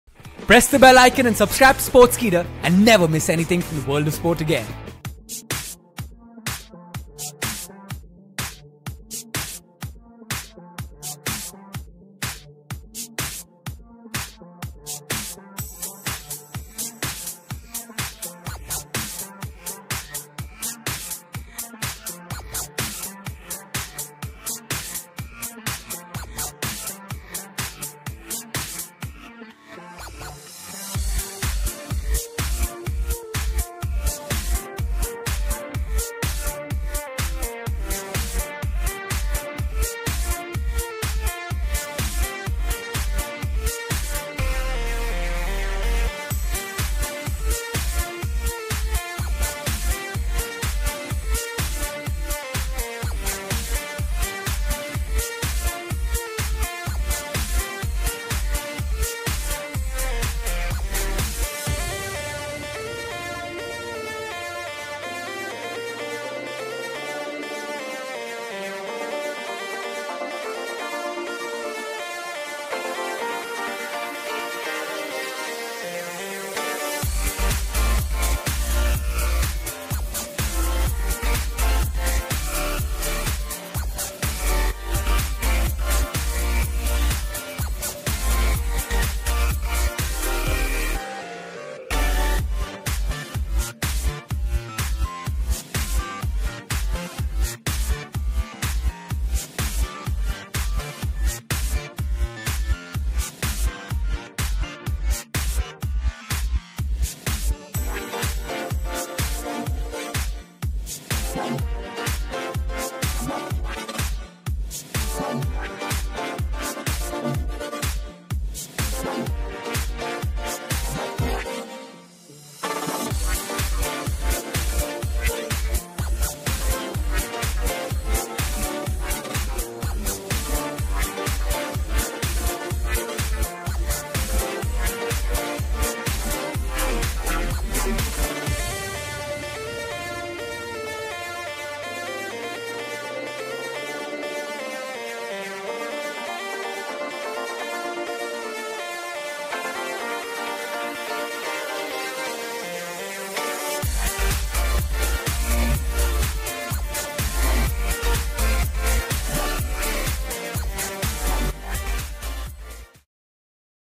Jose Mourinho vs Antonio Conte | Manchester United vs Chelsea Preview | Sportskeeda
Old Trafford is the place to be on Sunday as Manchester United take on Chelsea in what may prove to be a 6-pointer in the race for the Champions League places. For Chelsea, this is first of two successive away Premier League games in Manchester and the one which they have a more realistic chance taking some points from.

The last time they met this season - at Stamford Bridge - ended up in a 1-0 to the home side thanks to a header from Alvaro Morata. Both of them will be looking for a big result after draws against Spanish sides in the Champions League midweek, although Chelsea will be arguably the buoyant side having held the mighty Barcelona to a draw.